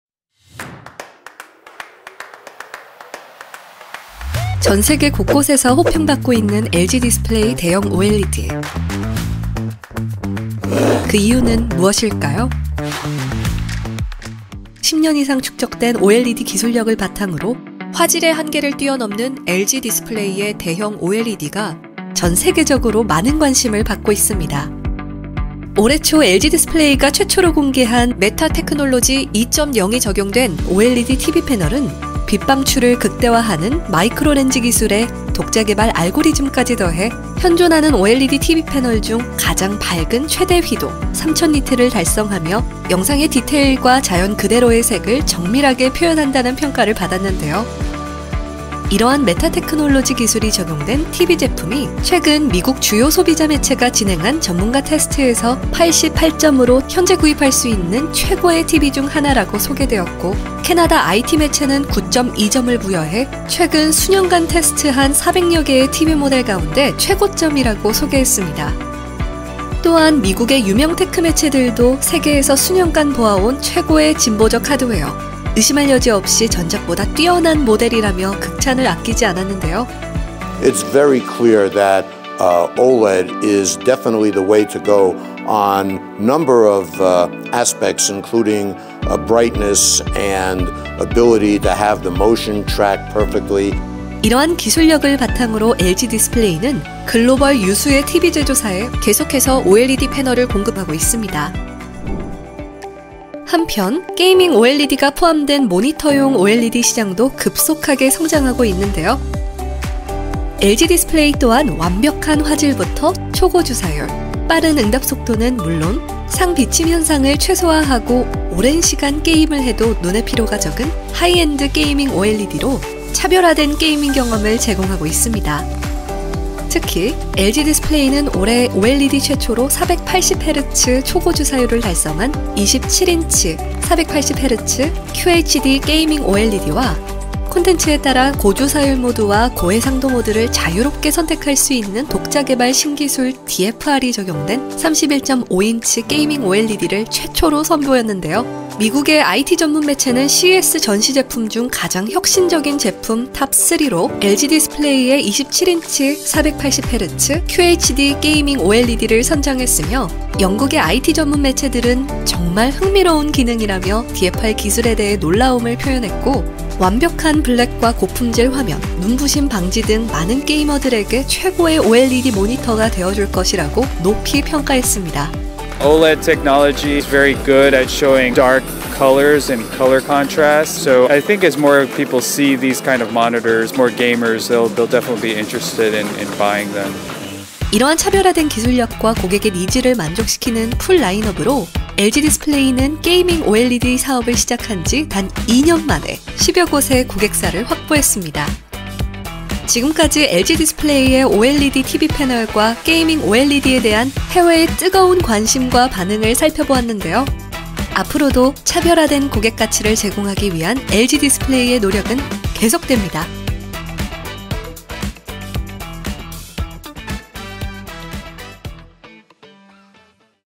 "One of the best TVs ..." 글로벌 호평이 쏟아지는 OLED TV와 게이밍 OLED!
'현재 구입할 수 있는 최고의 TV 중 하나!'
'CES 전시 제품 중 가장 혁신적인 제품 Top 3!'

다양한 디스플레이 혁신 기술로
전 세계 곳곳에서 호평 받고 있는
LG디스플레이의 OLED TV 패널과 게이밍 OLED!

🖥️ OLED TV 패널
메타 테크놀로지 2.0이 적용되어
빛 방출을 극대화하는 마이크로 렌즈 기술에
독자 개발 알고리즘까지 더해
현존 OLED TV 패널 중 가장 밝은 최대 휘도 3,000니트 달성!

🎮 게이밍 OLED
완벽한 화질부터 OLED 최초 480Hz 초고주사율,
빠른 응답속도는 물론 상 비침 현상을 최소화하고,
오랜 시간 게임을 해도 눈의 피로가 적은
'하이엔드 게이밍 OLED’로 몰입감 넘치는 게이밍 경험 제공!

앞으로도 차별화된 고객가치를 제공하기 위한
LG디스플레이의 노력은 계속됩니다!